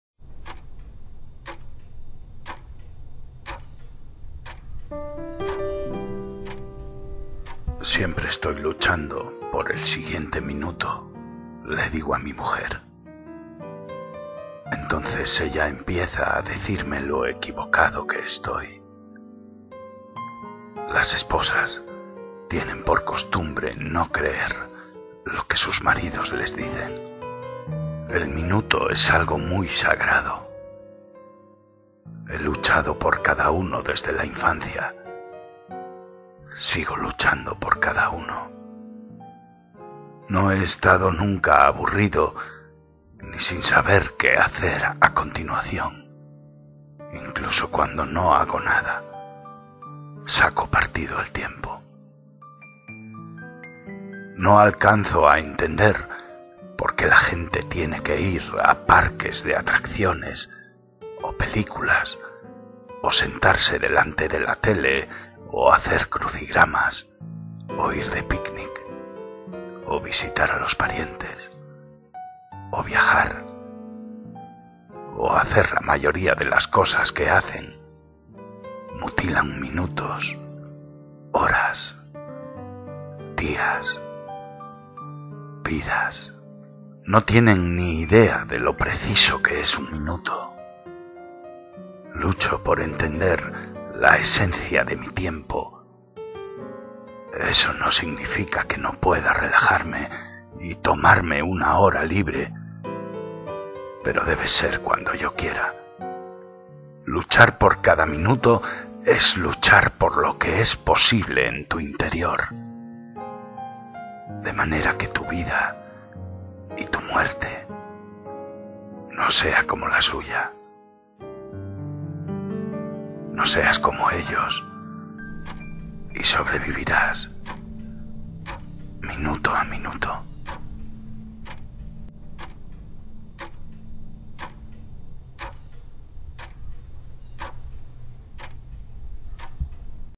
EL MINUTO. Charles Bukowski.
Siempre estoy luchando por el siguiente
minuto, le digo a mi mujer.
entonces ella empieza a decirme
lo equivocado que estoy.
las esposas tienen por costumbre
no creer lo que sus maridos
les dicen.

el minuto es algo muy
sagrado.
he luchado por cada uno desde la
infancia.
sigo luchando por cada uno.
no he estado nunca aburrido ni
sin saber qué hacer a continuación.
incluso cuando no hago nada
saco partido al tiempo.

no alcanzo a entender
por qué la gente tiene que ir
a parques de atracciones o películas
o sentarse delante de la tele
o hacer crucigramas
o ir de picnic
o visitar a los parientes
o viajar
o hacer la mayoría de las cosas
que hacen.
mutilan minutos
horas,
días,
vidas.

no tienen ni idea de lo
precioso que es un
minuto.

lucho por entender la esencia
de mi tiempo.
eso no significa que
no pueda relajarme
y tomarme una hora libre
pero debe de ser cuando
yo quiera.

luchar por cada minuto es
luchar por lo que es posible en
tu interior,
de manera que tu vida y tu muerte
no sea como la suya.

no seas como ellos
y sobrevivirás.

minuto a
minuto.